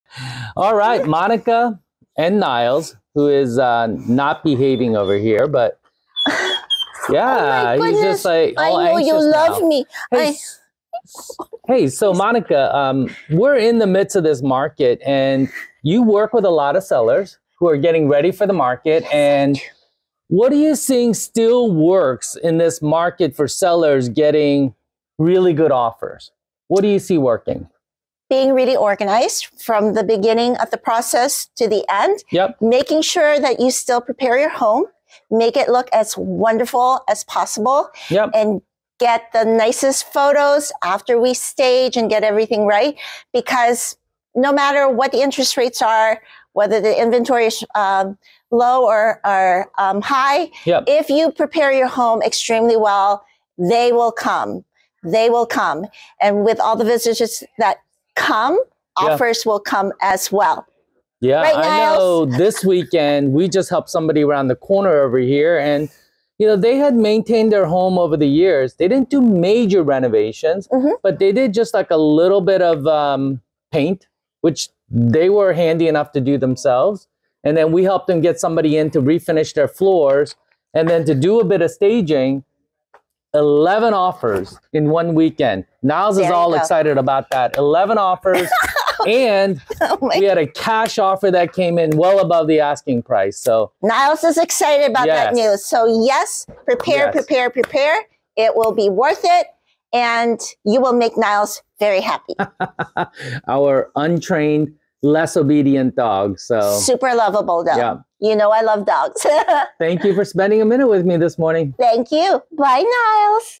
Preparation is Everything When it Comes to Selling Your Home
Join Jay and Monica as she shares insider tips for selling your home in today's market, accompanied by the lovable labradoodle Nyles! Learn the importance of preparation, staging, and making your home irresistible to potential buyers.